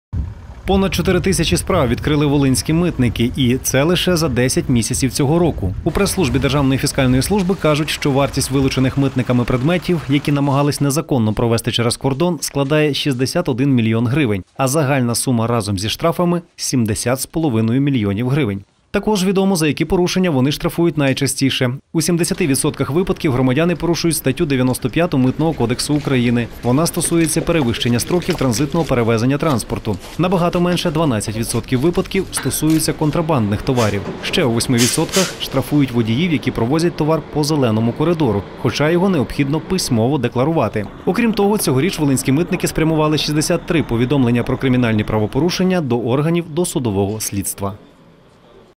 За що штрафують митники?
Понад чотири тисячі справ відкрили волинські митники. І це лише за десять місяців цього року. Представники Державної фіскальної служби кажуть, що вартість вилучених митниками предметів, які намагались незаконно провезти через кордон, складає 61 мільйон гривень. А загальна сума разом зі штрафами – 70 з половиною мільйонів гривень.

Також відомо, за які порушення вони штрафують найчастіше.
У 70% випадків громадяни порушують статтю 95 Митного кодексу України. Вона стосується перевищення строків транзитного перевезення транспорту. Набагато менше – 12% випадків – стосуються контрабандних товарів. Ще у 8% штрафують водіїв, які провозять товар по "зеленому коридору", хоча його необхідно письмово декларувати.
Окрім того, цьогоріч волинські митники спрямували 63 повідомлення про кримінальні правопорушення до органів досудового слідства.